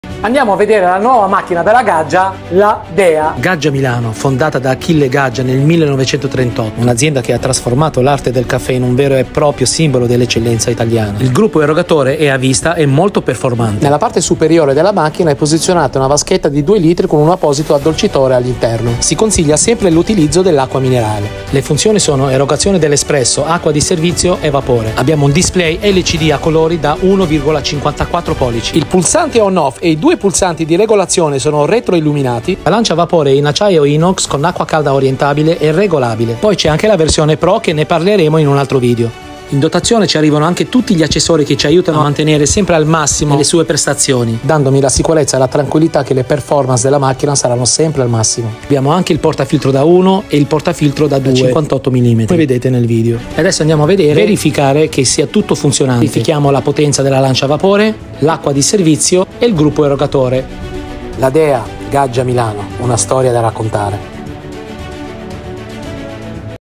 Gaggia Milano. “LA DEA”
LA DEA Gaggia Milano
Una storia da raccontare.
1. 💡LA DEA  unisce tradizione e innovazione per offrire un’esperienza di un estrazione come al bar, garantendo una distribuzione uniforme dell’acqua  mantenendo sempre stabile la temperatura per un espresso perfetto in ogni momento

2. 💝LA DEA grazie alla sua tecnologia avanzata, offre una versatilità straordinaria, permettendo di migliorare le tue skills di barista direttamente a casa secondo gli obbiettivi

3. ✅LA DEA di permetterà di degustare un espresso a regola d’arte ogni mattina e le tue giornate saranno ricche di energia e gusto, pronte ad affrontare ogni sfida con un sorriso.

Un ringraziamento speciale va:

🎗️Federico Tonti  (Sales Director Gaggia)per aver reso possibile tutto questo

✨Riccardo Tamburelli  (Sales Manager delle Macchine dell’espresso Gaggia) per avermi spiegato in maniera dettagliata la parte tecnica e funzionale della macchina.